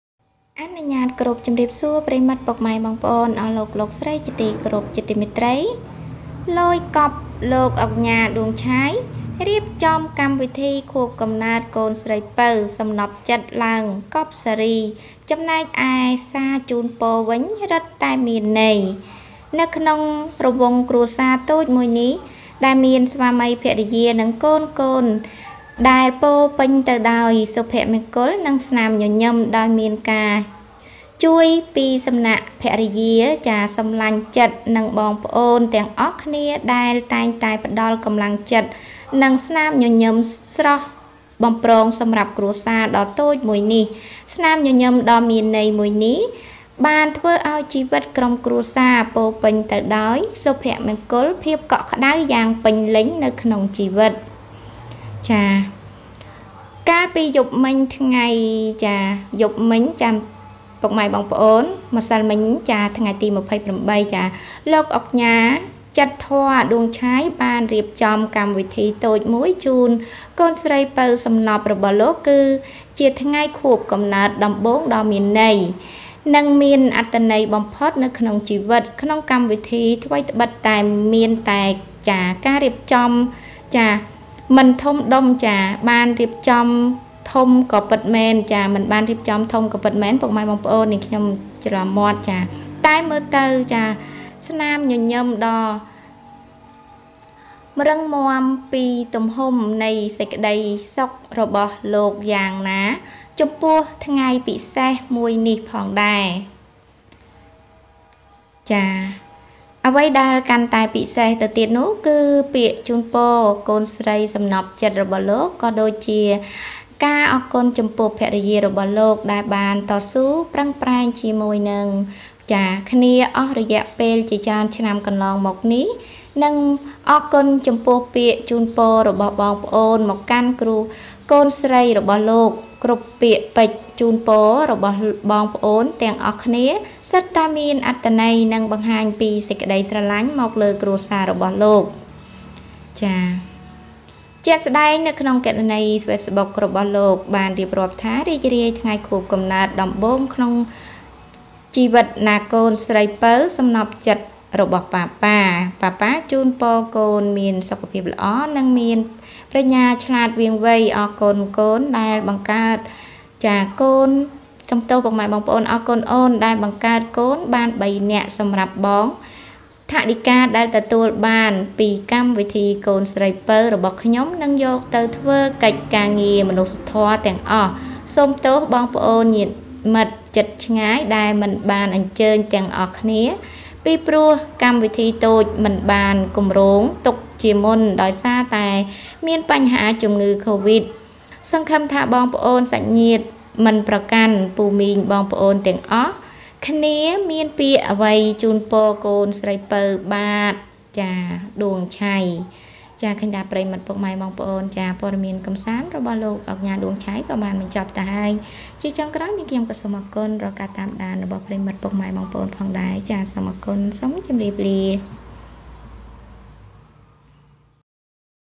ឡូយកប់! លោកឧកញ៉ា ឌួង ឆាយ រៀបចំកម្មវិធីខួបកំណើត កូនស្រីពៅសំណព្វចិត្ត ឡើងកប់ស៊េរី ចំណែកសារជូនពរវិញ រឹត
លូ យ កប់!  Duke Duong Chhay organized a birthday party for his favorite youngest daughter to bury the series, while the greeting message was even more meaningful ...

ឡូយកប់! លោកឧកញ៉ា ឌួង ឆាយ រៀបចំកម្មវិធីខួបកំណើត កូនស្រីពៅសំណព្វចិត្ត ឡើងកប់ស៊េរី ចំណែកសារជូនពរវិញ រឹតតែមានន័យ...

channel/KhmerNewsToday